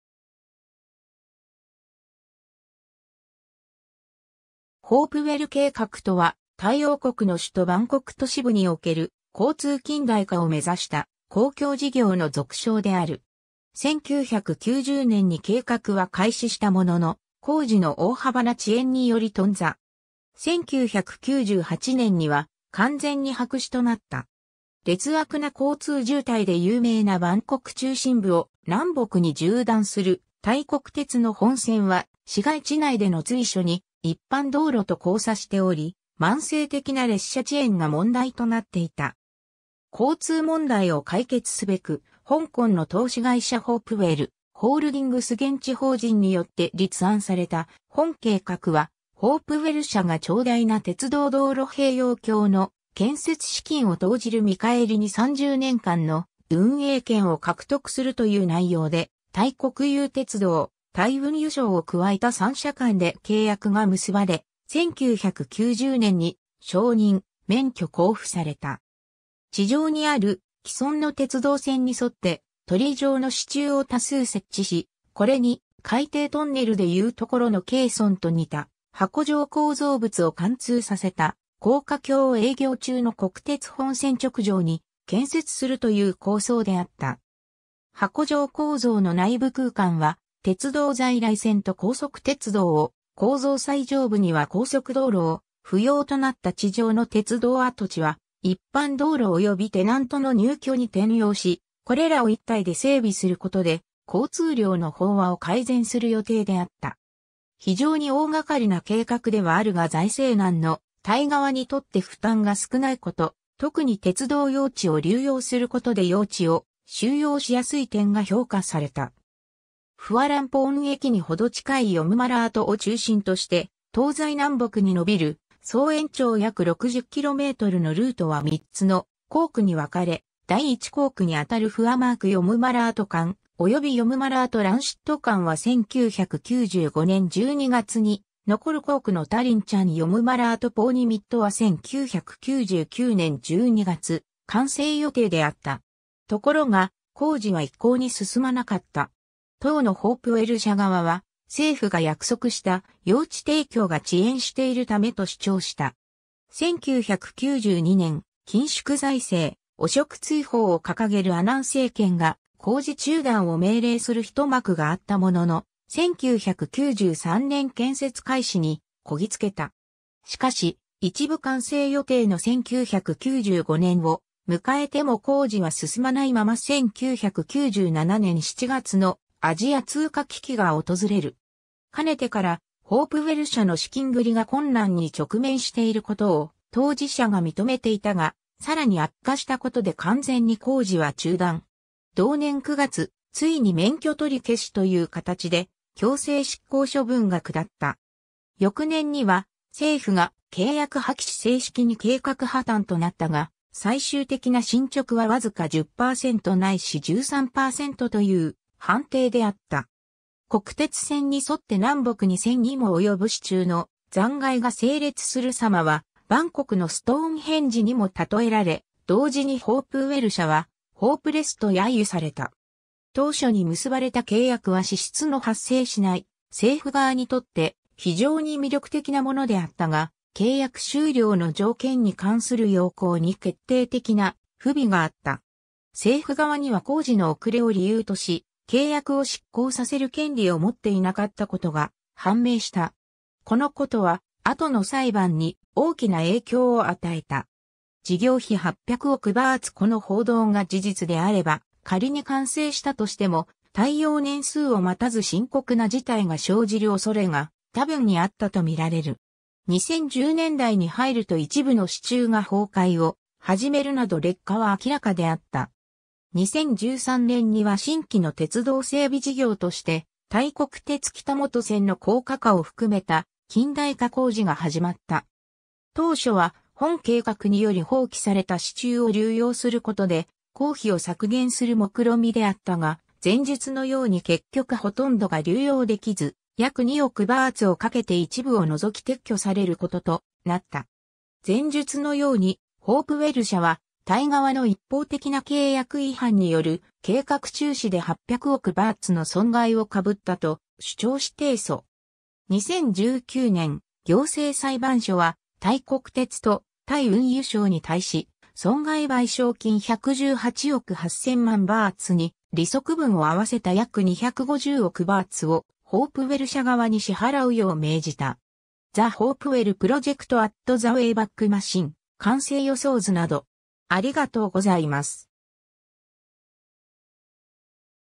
ホープウェル計画
ホープウェル計画（ホープウェルけいかく）とは、タイ王国の首都バンコク都市部における交通近代化を目指した公共事業の俗称である。
（英名：”Bangkok Elevated Road and Train System”、略称”BERTS”）1990年に計画は開始したものの、工事の大幅な遅延により頓挫。
1998年には完全に白紙となった。
 劣悪な交通渋滞で有名なバンコク中心部を南北に縦断するタイ国鉄の本線は、市街地内での随所に一般道路と交差しており、慢性的な列車遅延が問題となっていた。
 交通問題を解決すべく香港の投資会社ホープウェル・ホールディングス（合和実業）現地法人（以下、親会社・現地法人併せてホープウェル社と略す）によって立案された本計画は、ホープウェル社が長大な鉄道道路併用橋の建設資金を投じる見返りに30年間の運営権を獲得するという内容で、タイ国有鉄道、タイ運輸省を加えた三者間で契約が結ばれ（この時の契約内容が後に問題となる）、1990年に承認、免許交付された。
 地上にある既存の鉄道線に沿って鳥居状の支柱を多数設置し、これに海底トンネルでいうところのケーソンと似た箱状構造物を貫通させた高架橋を営業中の国鉄本線直上に建設するという構想であった。
箱状構造の内部空間は鉄道在来線と高速鉄道を、構造最上部には高速道路を、不要となった地上の鉄道跡地は一般道路およびテナントの入居に転用し、これらを一体で整備することで交通量の飽和を改善する予定であった。
非常に大掛かりな計画ではあるが財政難のタイ側にとって負担が少ないこと、とくに鉄道用地を流用することで用地を収用しやすい点が評価された。
 フワランポーン駅にほど近いヨムマラートを中心として東西南北に伸びる総延長約60kmのルートは３つの工区に分かれ、第一工区にあたるフアマーク-ヨムマラート間、およびヨムマラート-ランシット間は1995年12月に、残る工区のタリンチャン - ヨムマラート - ポーニミットは1999年12月完成予定であった。
 ところが、工事は一向に進まなかった。
当のホープウェル社側は政府が約束した用地提供が遅延している為と主張した。
 1992年、緊縮財政・汚職追放を掲げるアナン政権が工事中断を命令する一幕があったものの、1993年建設開始にこぎつけた。
しかし、一部完成予定の1995年を迎えても工事は進まないまま1997年7月のアジア通貨危機が訪れる。
かねてからホープウェル社の資金繰りが困難に直面していることを当事者が認めていたが、更に悪化したことで完全に工事は中断。
同年9月、ついに免許取消という形で強制執行処分が下った。
翌年には政府が契約破棄し正式に計画破綻となったが、最終的な進捗はわずか10%ないし13%という判定であった。
国鉄線に沿って南北に千にも及ぶ支柱の残骸が整列する様は『バンコクのストーンヘンジ』にも例えられ、同時にホープウェル社は『ホープレス』と揶揄された。
 当初に結ばれた契約は支出の発生しない政府側にとって非常に魅力的なものであったが、契約終了の条件に関する要項に決定的な不備があった。
政府側には工事の遅れを理由とし契約を失効させる権利を持っていなかったことが判明した。
このことは後の裁判に大きな影響を与えた。
 事業費800億バーツ（当時レートで32億USドル)という巨大プロジェクトの実態、たとえば楽観的に見積もられた費用対効果や工期といった杜撰な計画が後に露呈。契約に関する不正の噂もつきまとった。 北本線沿いに残された支柱 2005年の段階で、建設された支柱の耐荷重は計画値を満たしていなかった可能性が高いことが報じられている。（予定の約30%にも満たない水準）この報道が事実であれば、仮に完成したとしても耐用年数を待たず深刻な事態が生じる恐れが多分にあったとみられる。
 2010年代に入ると（鉄骨剥き出しで放置されていたとはいえ）一部の支柱が崩壊を始めるなど劣化は明らかであった。
 2013年には新規の鉄道整備事業としてタイ国鉄北本線の高架化を含めた近代化工事が始まった。
当初は本計画により放棄された支柱を流用することで工費を削減する目論みであったが、前述のように結局ほとんどが流用できず、約2億バーツをかけて一部を除き撤去されることとなった。
 前述のように、ホープウェル社はタイ側の一方的な契約違反による計画中止で800億バーツの損害を被ったと主張し提訴。
 2019年、行政裁判所はタイ国鉄とタイ運輸省に対...